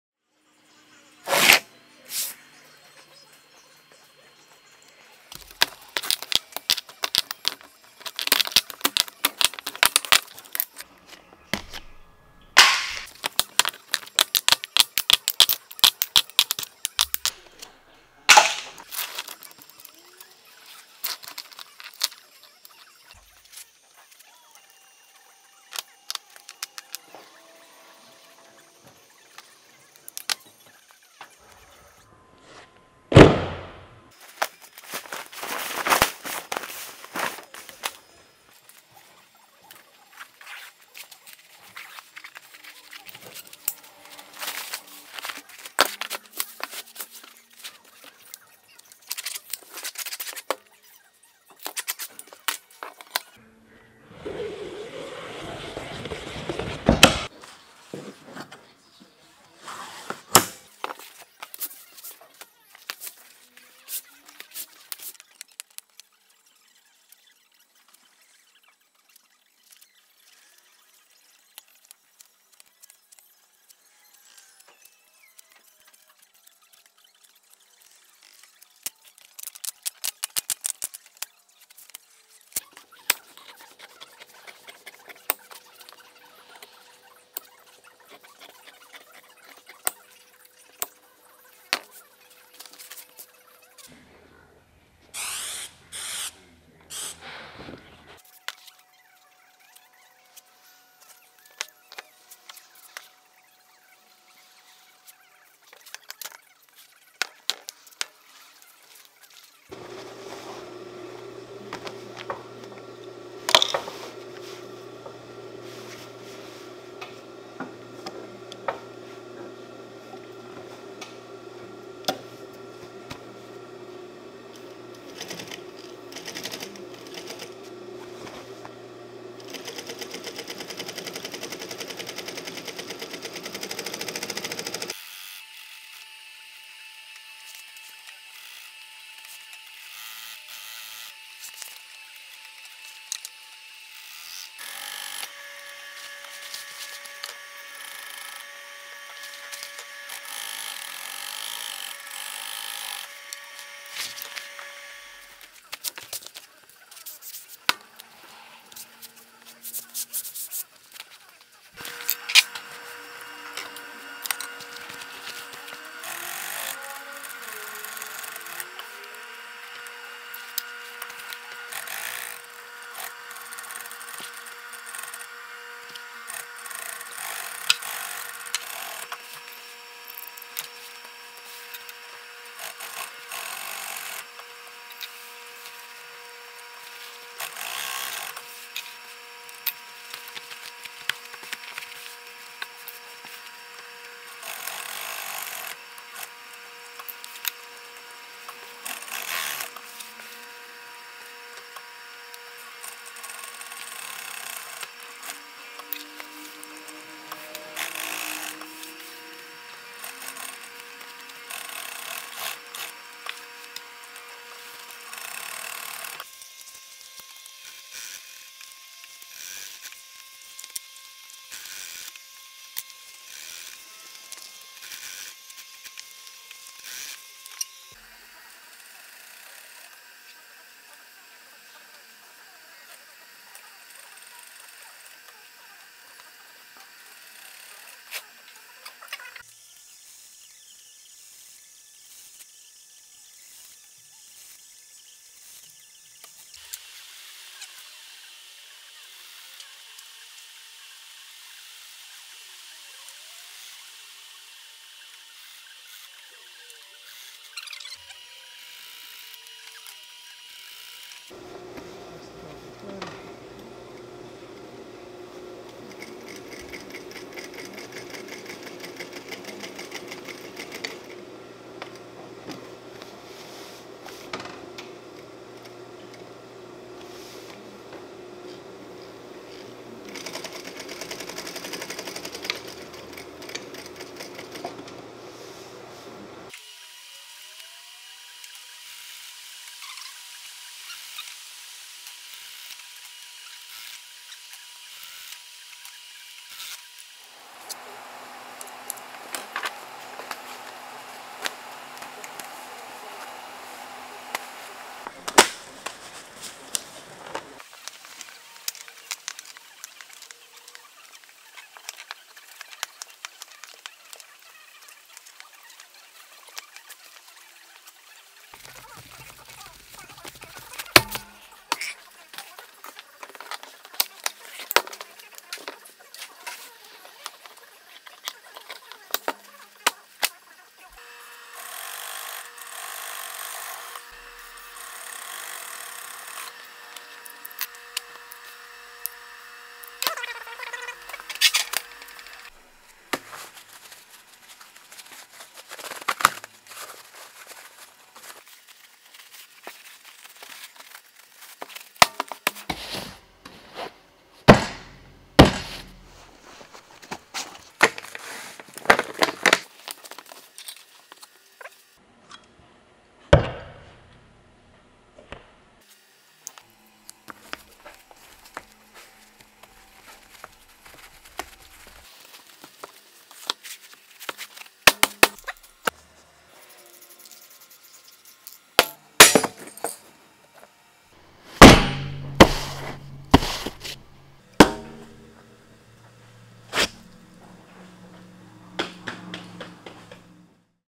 DiResta Wrench Storage Roll up
Watch me use an old waxed canvas tarp to make a wrench roll up to keep in my truck !
I am using a juki industrial machine I got on craigslist.

for voiced over videos and exclusive content